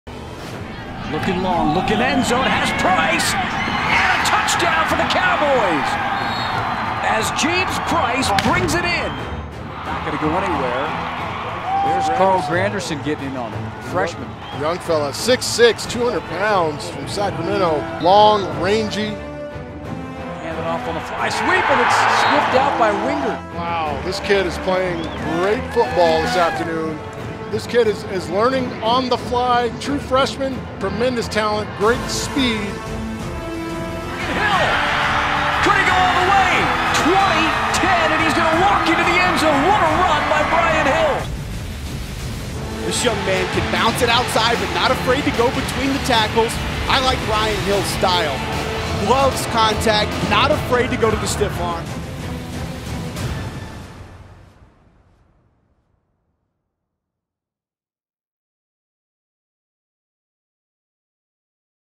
Cowboy Football Signing Day 2016 Promo
Live stream of Cowboy Football's 2016 signing class is available on GoWyo.com on Wednesday, February 3rd at 3 PM MT.

Then head over to the Marian H. Rochelle Gateway Center for a public event at 5:30pm!